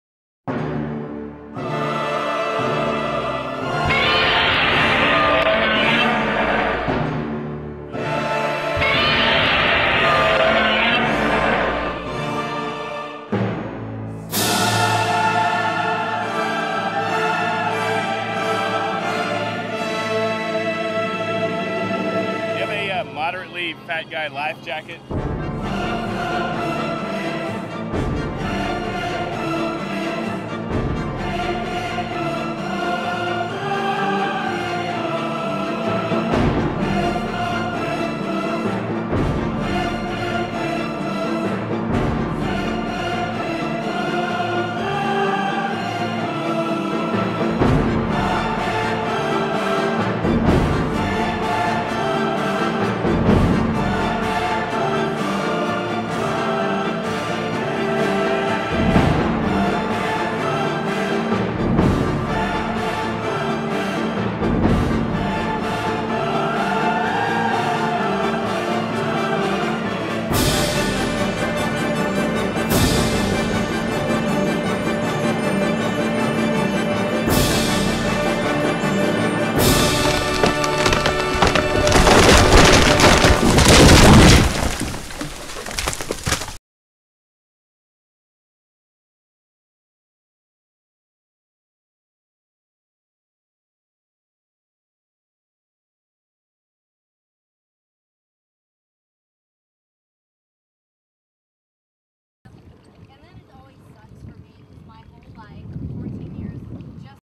Fat Guy Wipeout!
Deb took Jason surfing out on the lake.  What's funnier than seeing a fat guy try to get up on a board?  Seeing him get up and then fall down.